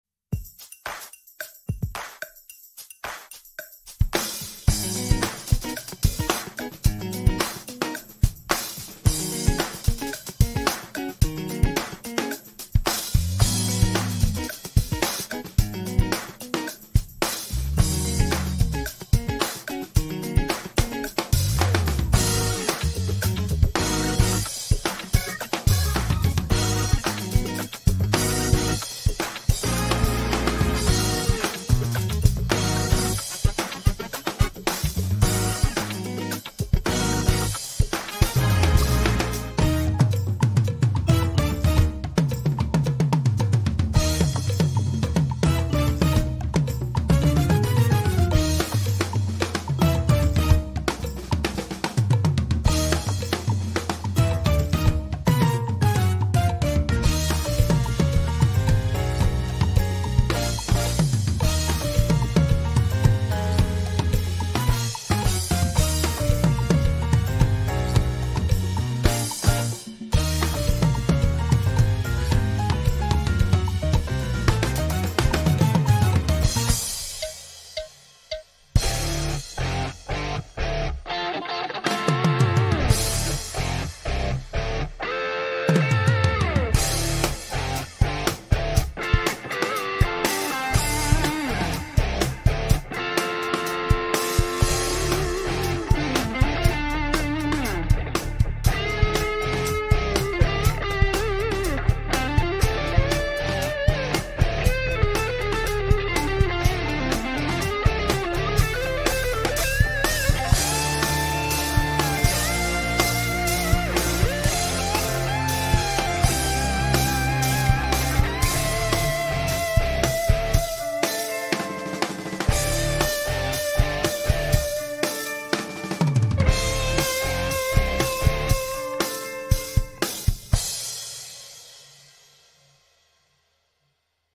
Bewerbungsvideo DFS 2021 - Joen Abduloski
Joen Abduloski machte wieder am diesjährigen Drum Festival Switzerland.